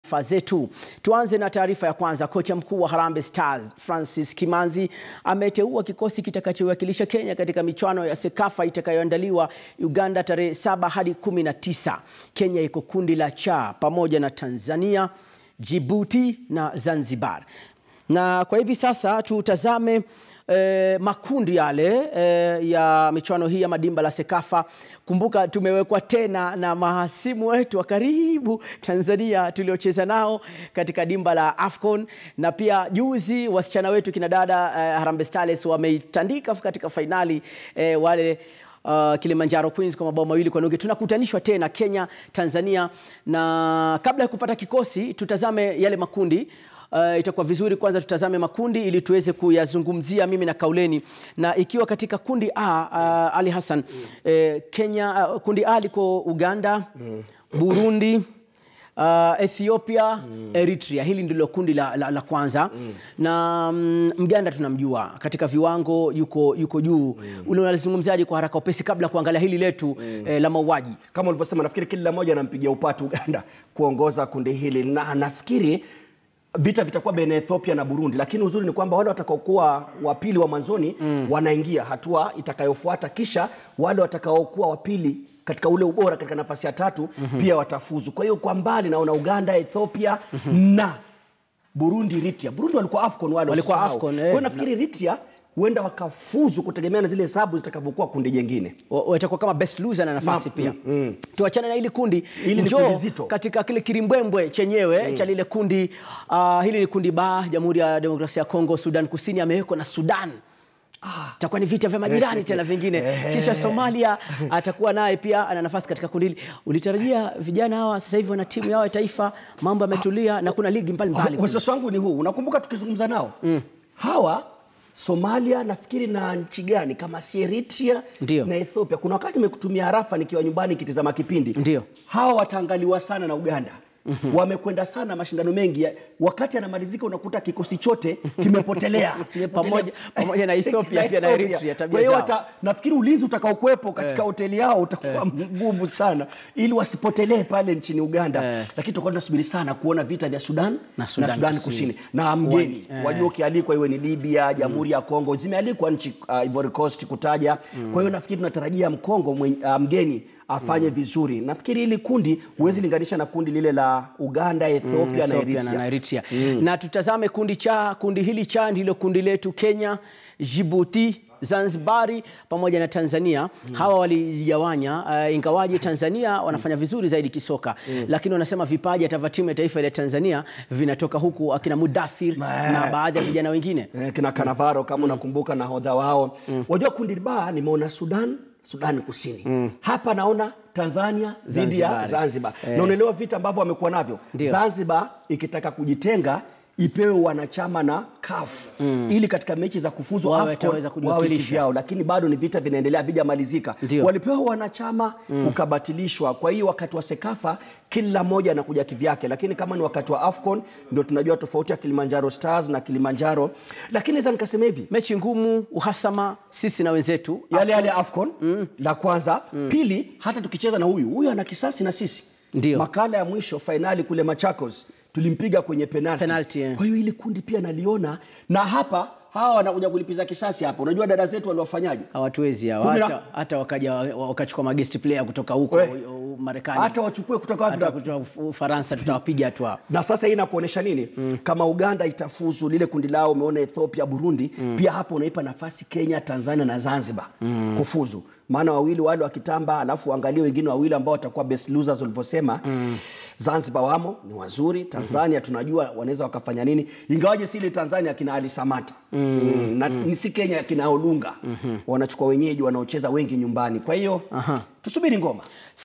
Francis Kimanzi ateua kikosi kitakachowakilisha Kenya katika michuano ya CECAFA
Kocha mkuu wa Harambee Stars Francis Kimanzi ameteuwa kikosi kitakachowakilisha Kenya katika michuano ya CECAFA itakayoandaliwa Uganda kuanzia tarehe 7 hadi 19. Kenya iko kundi la C pamoja na Tanzania,Djibouti na Zanzibar.